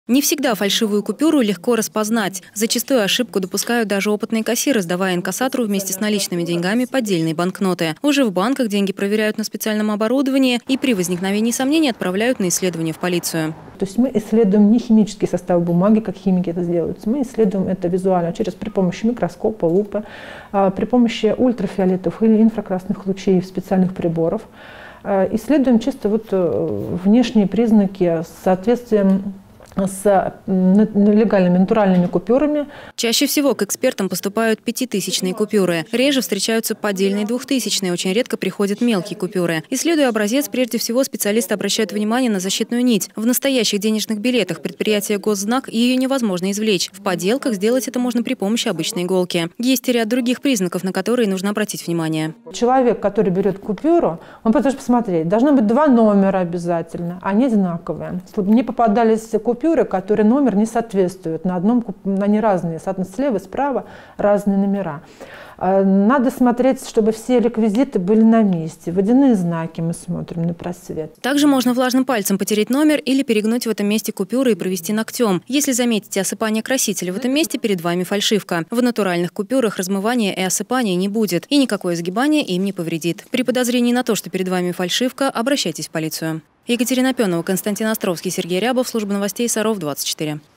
Как отличить фальшивые деньги от настоящих?
Рост числа фальшивых купюр в России отмечают эксперты. Какие банкноты чаще подделывают, и как отличить их от настоящих?